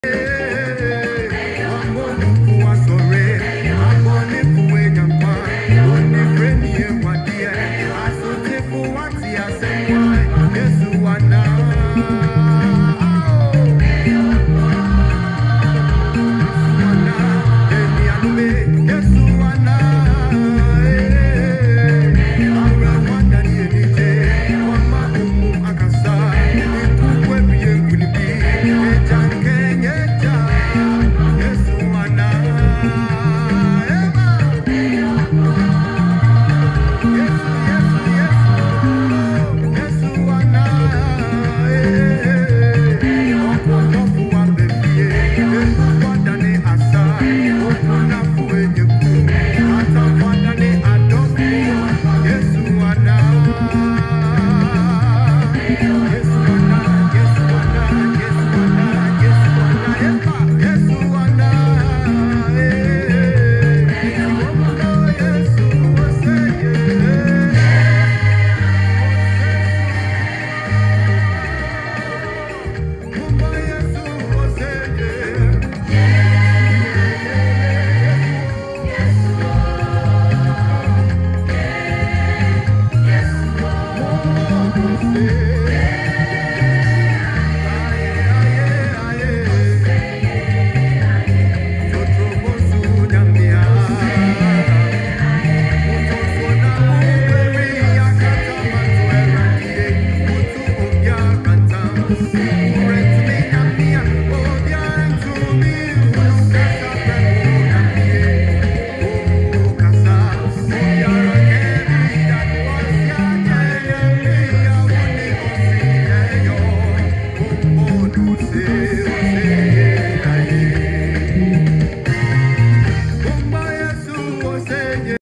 The Last baseline is the best.. watch to the end (Practice Session) Ghanaian Pentecostal Praise
The group chat is the only place you can request songs and get access to private and group lessons.